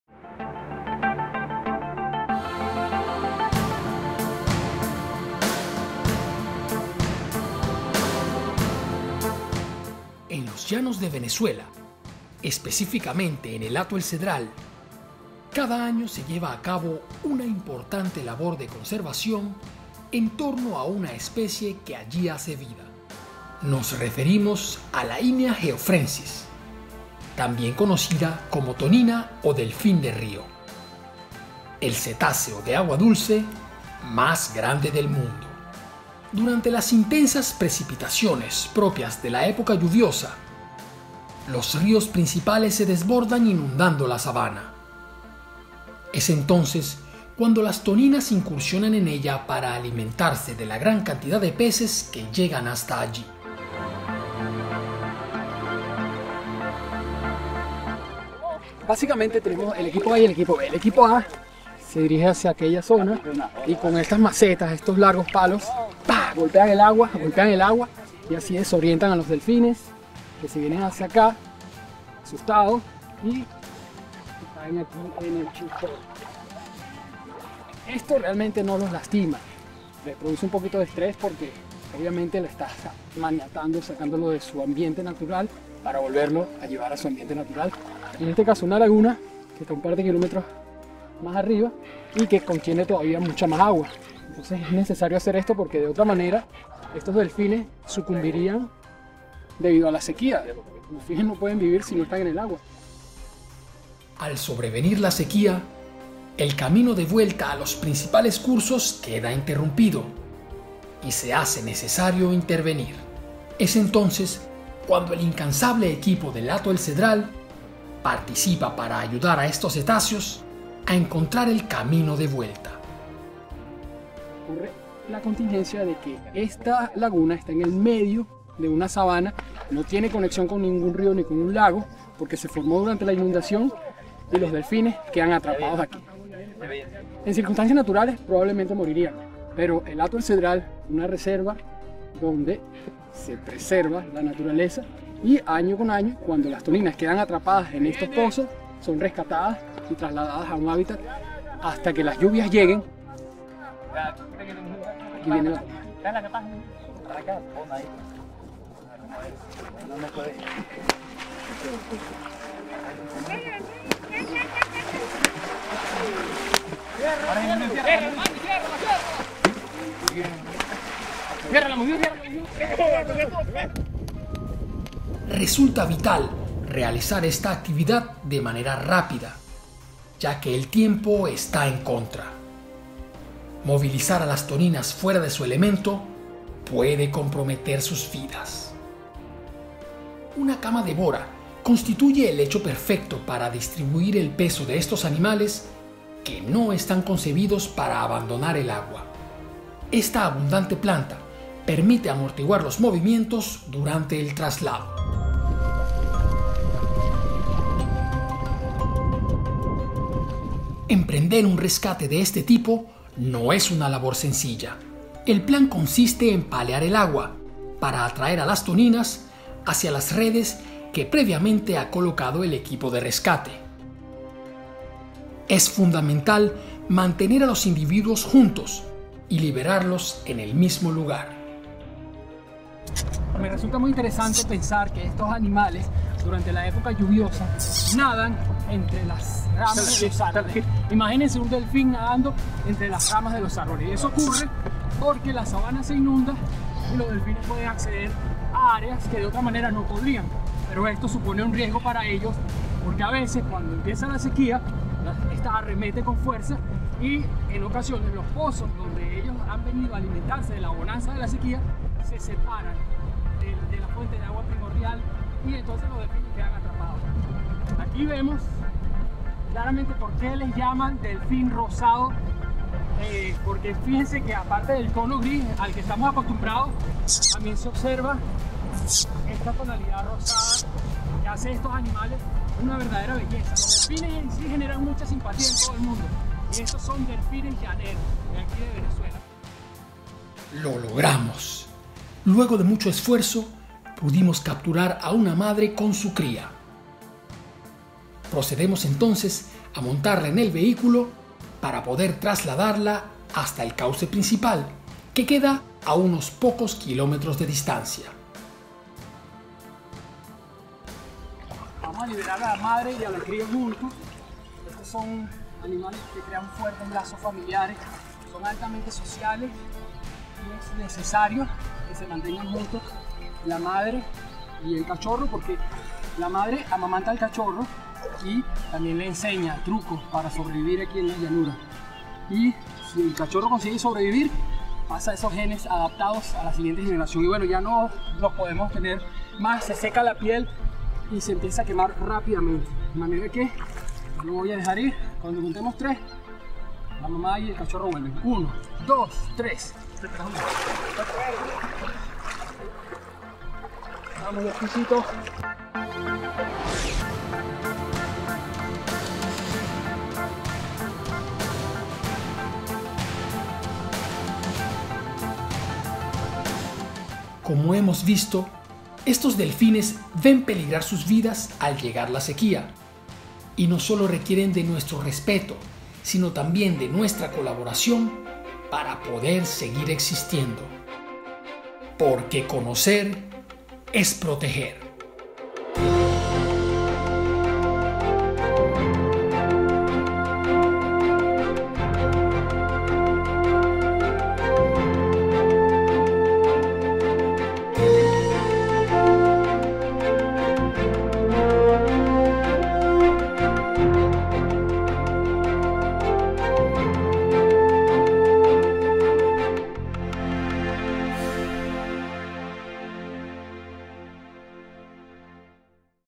Rescate de toninas en el Hato El Cedral
Todos los años, cuando la sequía arrecia, las aguas descienden muy rapido dejando a alguna toninas atrapadas. Su muerte seria un hecho pero la gente de el Hato El Cedral, realiza una operación de rescate y salvamento para evitar que esto ocurra.